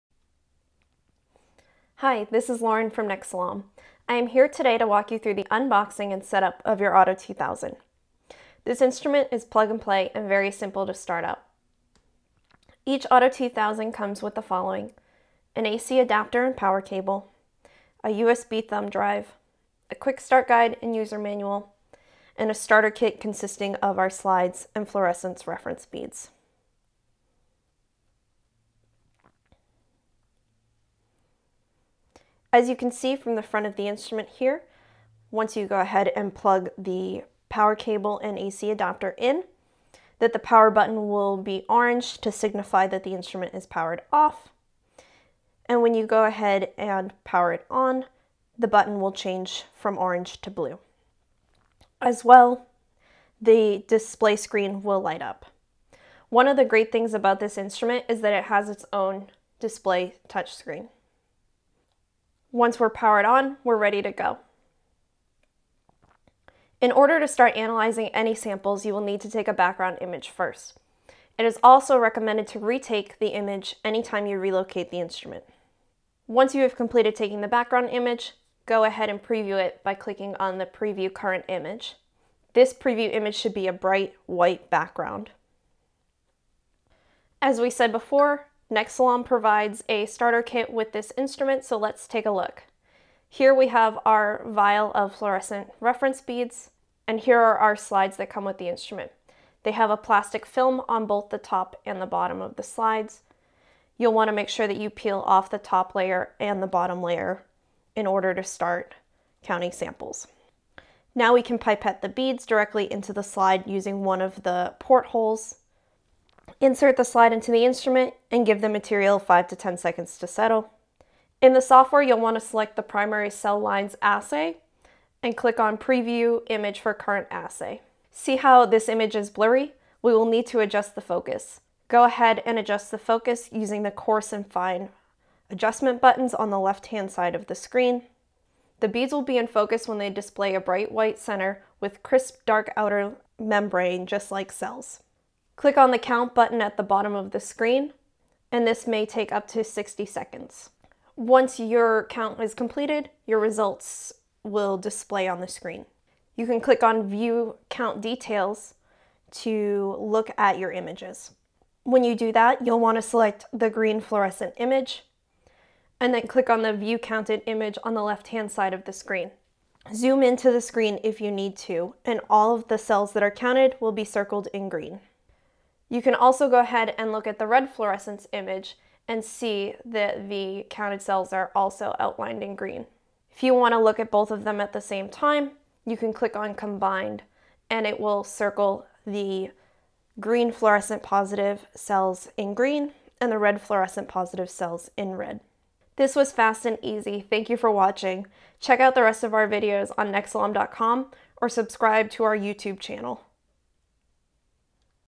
Getting Started with the Cellometer Auto 2000 Cell Counter
See how easy it is to setup and get started using the Cellometer Auto 2000 cell counter.